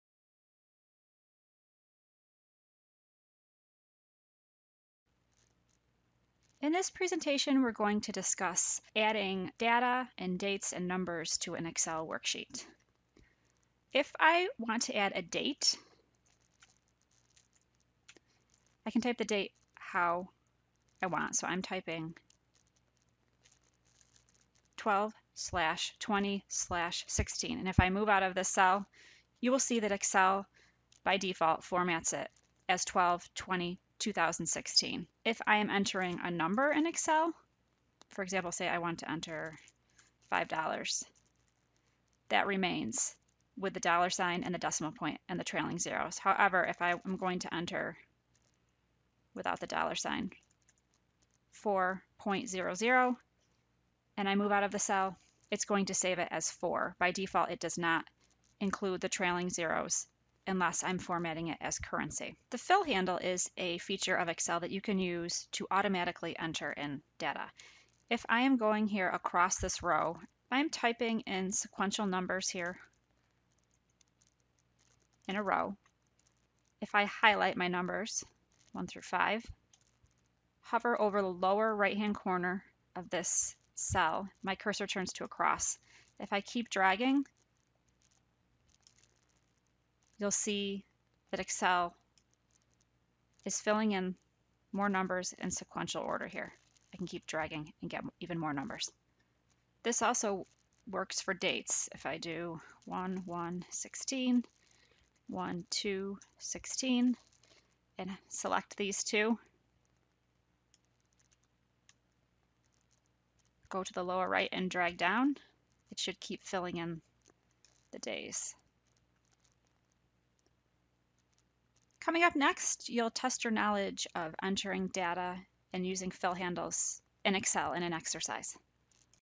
Entering Data and Using the Fill Handle
Learn how to add data and dates and numbers to an Excel worksheet.